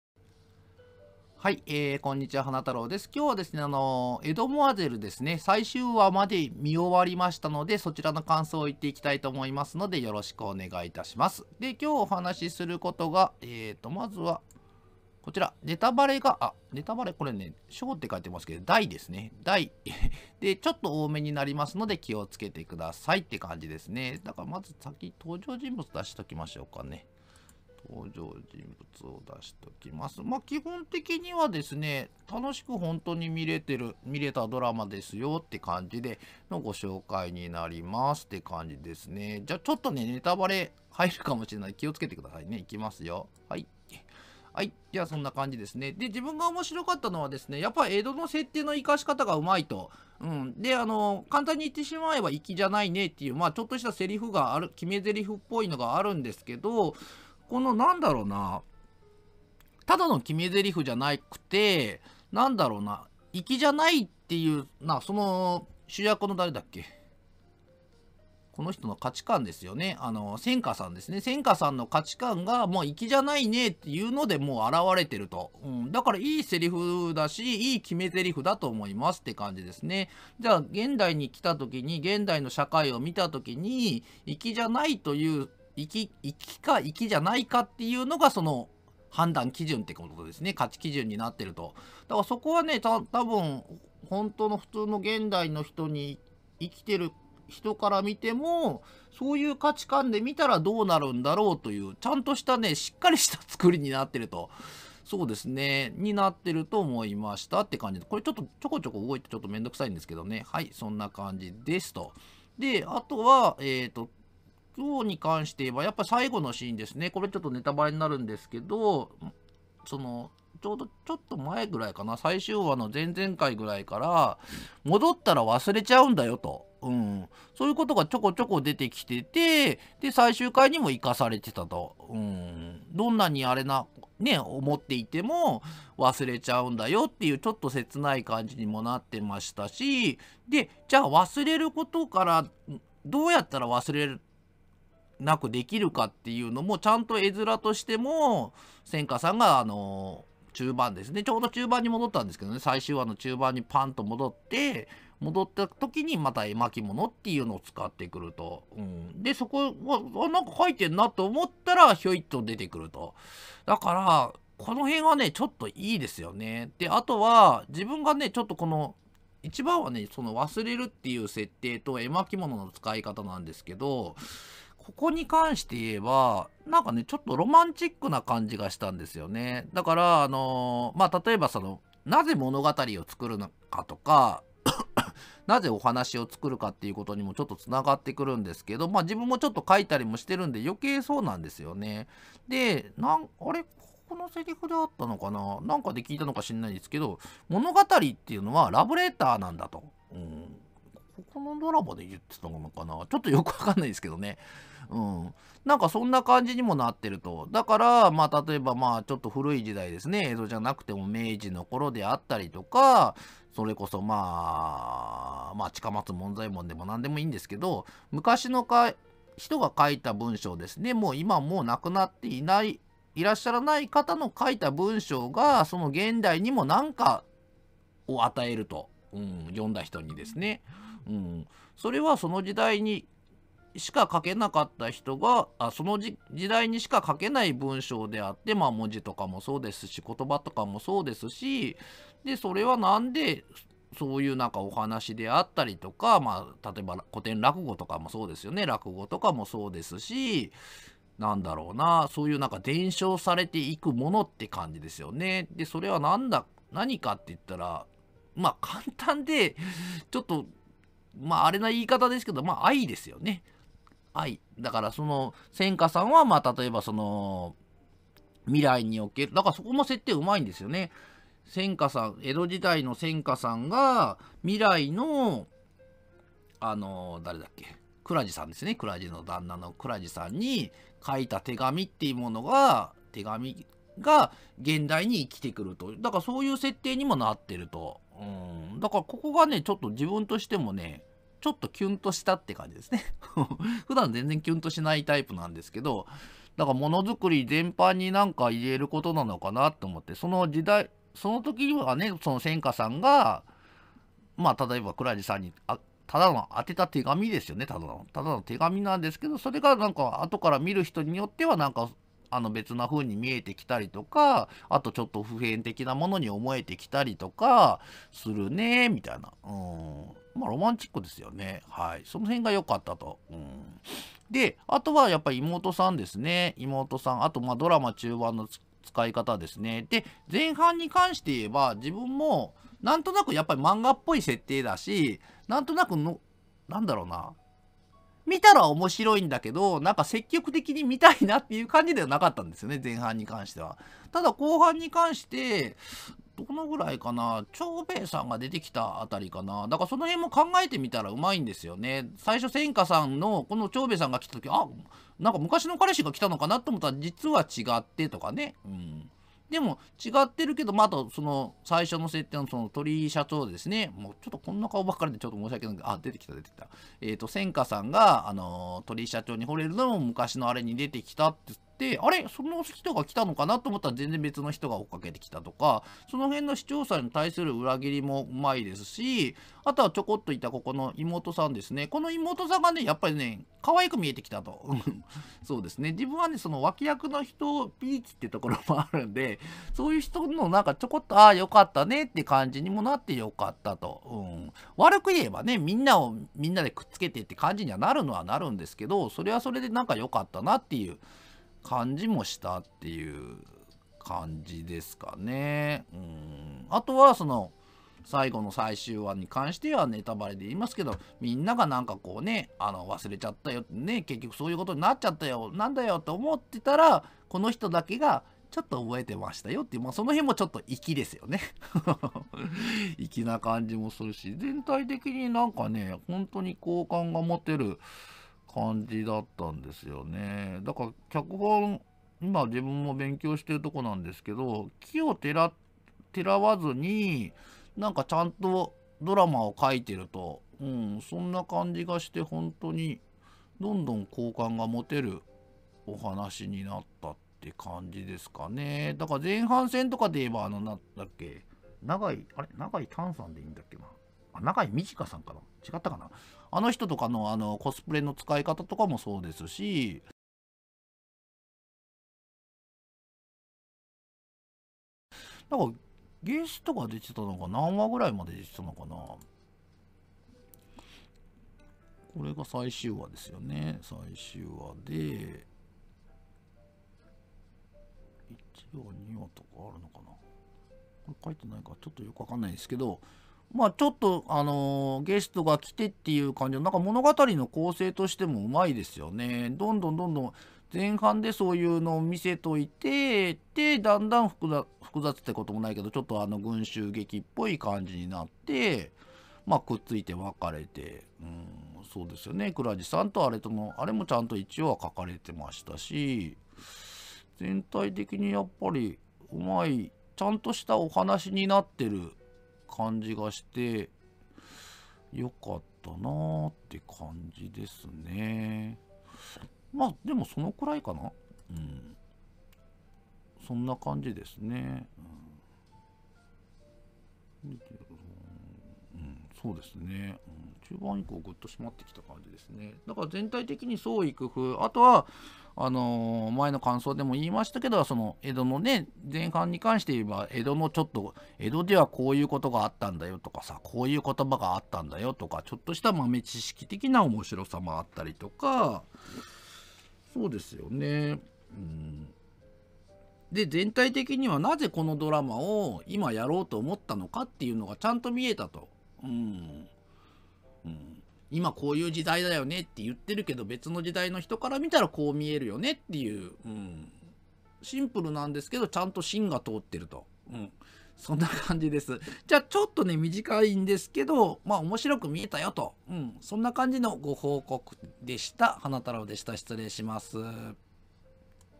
「江戸モアゼル」最終話感想。【#289】
江戸モアゼル 出演：岡田結実さん、葉山奨之さん、吉谷彩子さん、
前田公輝さん、田中直樹さん、山口まゆさん、森田甘路さん

この動画に出てくる映画

花たろうフリー(編集動画中心、多種多様な動画にチャンレンジ中です!!)